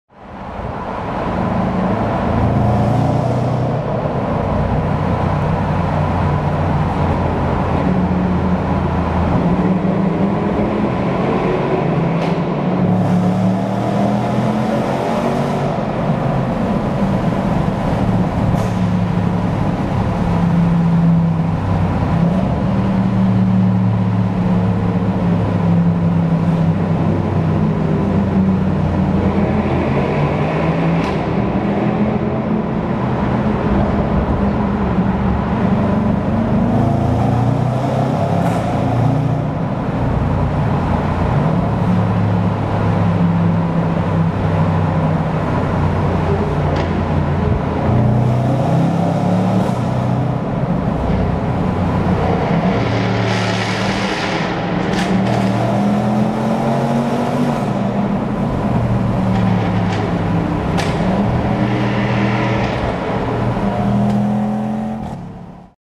Tunnel Sound Battle: Range Rover Sport SVR vs Jaguar F-type R AWD vs Mercedes SL63 AMG
Bit of a spontaneous sound battle of the V8s on the way back from Goodwood through Hindhead Tunnel!